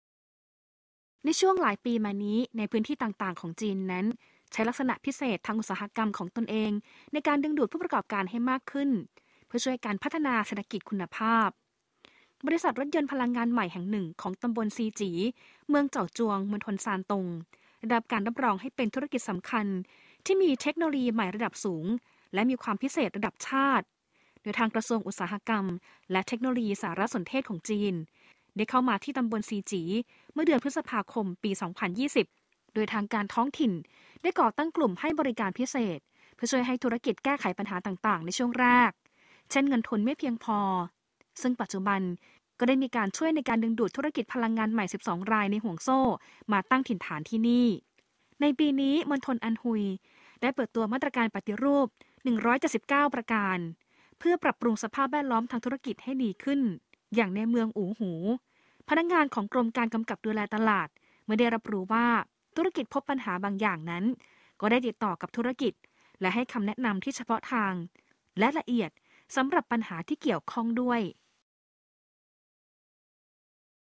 จีนปรับสภาพแวดล้อมทางธุรกิจ (แบบเสียงบรรยายไทย)
ในช่วงหลายปีมานี้ พื้นที่ต่าง ๆ ของจีนใช้ลักษณะพิเศษทางอุตสาหกรรมของตนเองในการดึงดูดผู้ประกอบการมากขึ้น เพื่อช่วยพัฒนาเศรษฐกิจคุณภาพ
บริษัทรถยนต์พลังงานใหม่แห่งหนึ่งของตำบลซีจี๋ เมืองเจ่าจวง มณฑลซานตง ได้รับการรับรองให้เป็นธุรกิจสำคัญที่มีเทคโนโลยีใหม่ระดับสูงและมีความพิเศษระดับชาติ โดยกระทรวงอุตสาหกรรมและเทคโนโลยีสารสนเทศจีน ซึ่งได้เข้ามาตำบลซีจี๋เมื่อเดือนพฤษภาคมปี 2020
ทางการท้องถิ่นได้ก่อตั้งกลุ่มให้บริการพิเศษ เพื่อช่วยให้ธุรกิจแก้ไขปัญหาต่าง ๆ ในช่วงแรก เช่น เงินทุนไม่เพียงพอ ปัจจุบัน ได้มีส่วนช่วยในการดึงดูดธุรกิจพลังงานใหม่12 รายในห่วงโซ่ มาตั้งที่นี่แล้ว
ปีนี้ มณฑลอันฮุยได้เปิดตัวมาตรการปฏิรูป 179 ประการ เพื่อปรับปรุงสภาพแวดล้อมทางธุรกิจให้ดีขึ้น ในเมืองอู๋หู พนักงานของกรมการกำกับดูแลตลาด เมื่อได้รับรู้ว่า ธุรกิจพบปัญหาบางอย่าง ก็ได้ติดต่อกับธุรกิจ และให้คำแนะนำที่เฉพาะทางและละเอียดสำหรับปัญหาที่เกี่ยวข้อง
คลิปข่าวจากภาคภาษาไทย ศูนย์เอเเชียแอฟริกา สถานีวิทยุและโทรทัศน์ส่วนกลางแห่งประเทศจีน (CMG)